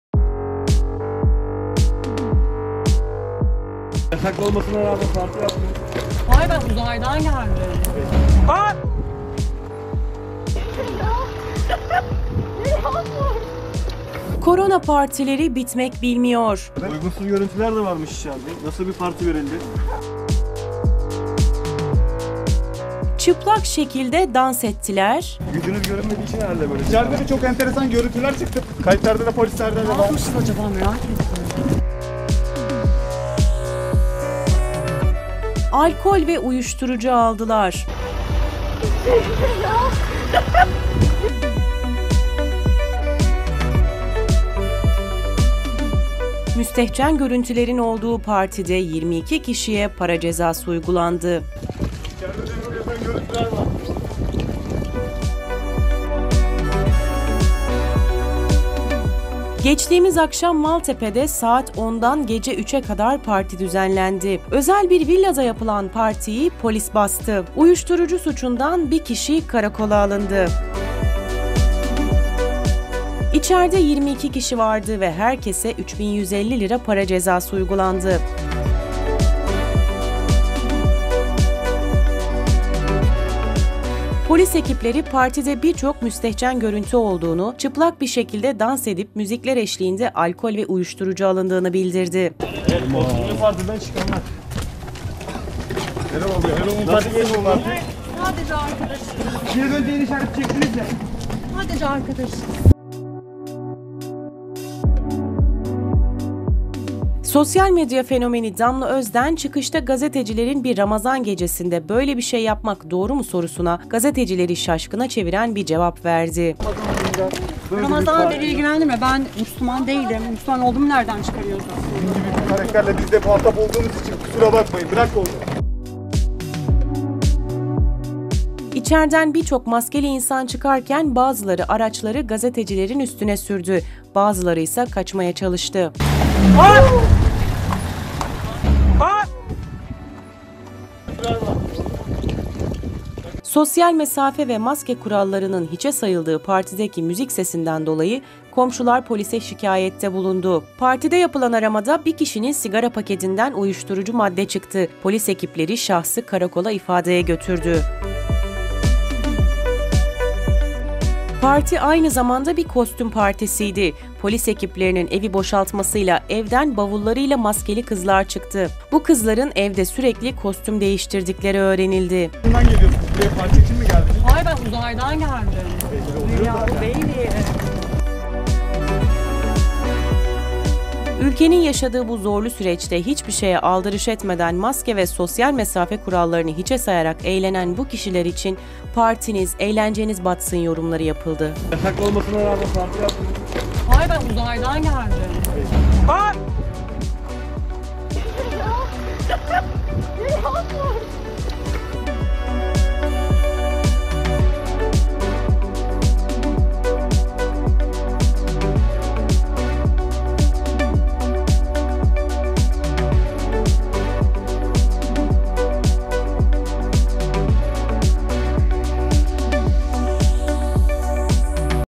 Damla Özden'in 'Sansürsüz' Partisine Polis Baskını | PANDEMİ VE RAMAZAN DİNLEMEDİLER
Dün Maltepe'de özel bir villada yapılan partiye polis baskını yapıldı. Damla Özden gibi birçok fenomen ismin olduğu partide uygunsuz görüntüler kaydedilirken polisler uyuşturucu madde ele geçirdi. Kızlı erkekli insanların Çıplak bir şekilde dans edip müzikler eşliğinde alkol ve uyuşturucu aldıklarını belirtildi. Fenomen Damla Özden çıkışta gazetecilerin "Ramazan gecesinde böyle bir şey yapmak doğru mu?" sorusuna "Ben Müslüman değilim ki size ne!" yanıtını verdi.